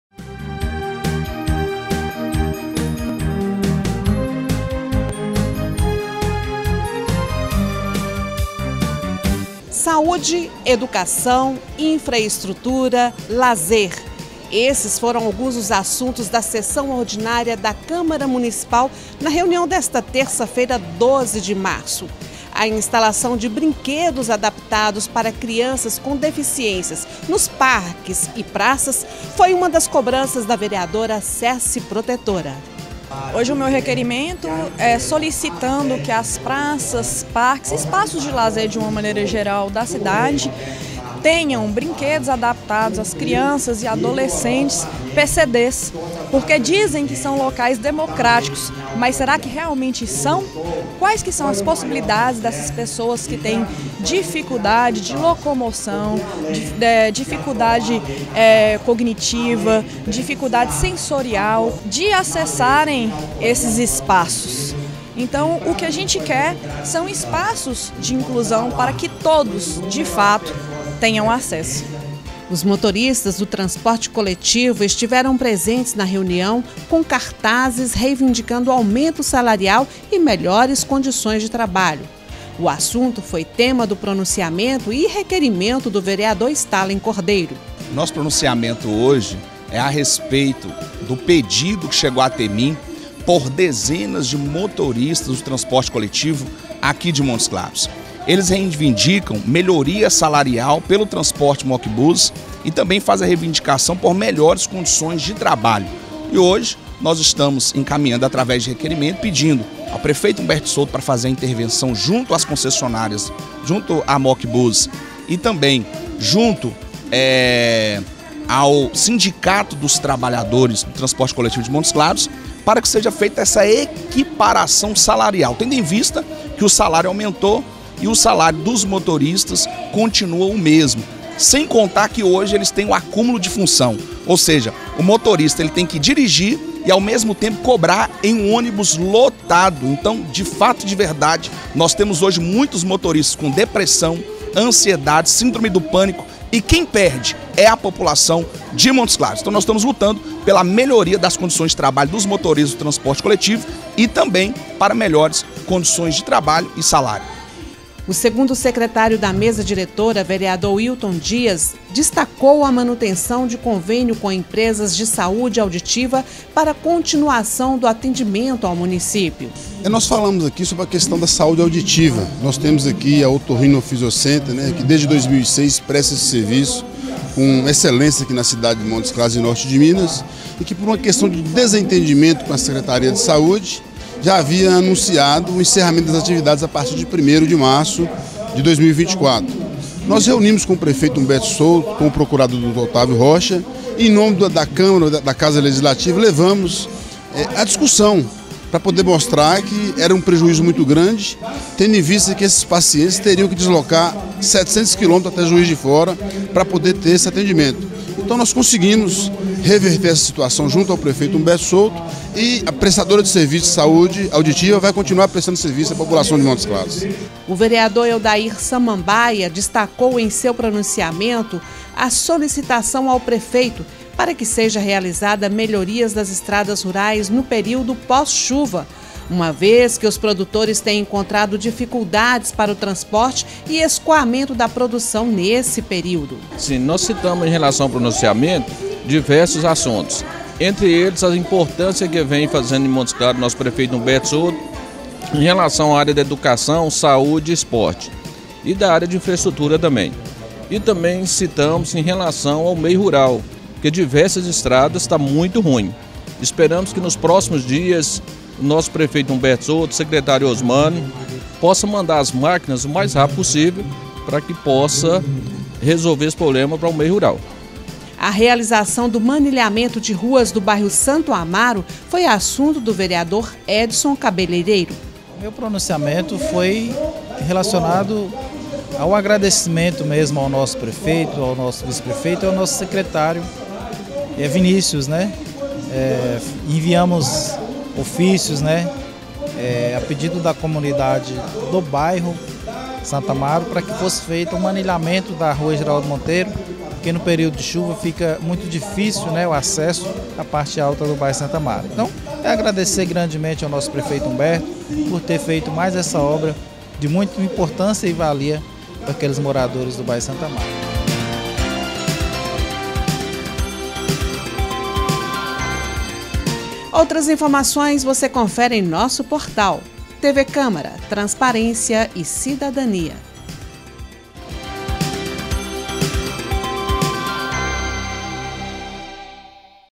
INFORME CÂMARA | SESSÃO ORDINÁRIA 12-03-2024
O Projeto de Lei 42/24 prevê a transferência de R$ 11,88 milhões do Município para nove instituições de educação, sem fins lucrativos, que apresentaram propostas para atender alunos do ensino infantil e da educação especial não atendidos pelos Sistemas municipal, estadual e particular de Ensino em Montes Claros.

Apresentado pelo Executivo municipal, o texto foi aprovado pela Câmara de Vereadores por unanimidade, em sessão ordinária nesta terça-feira (12/3), e agora segue para sanção do prefeito.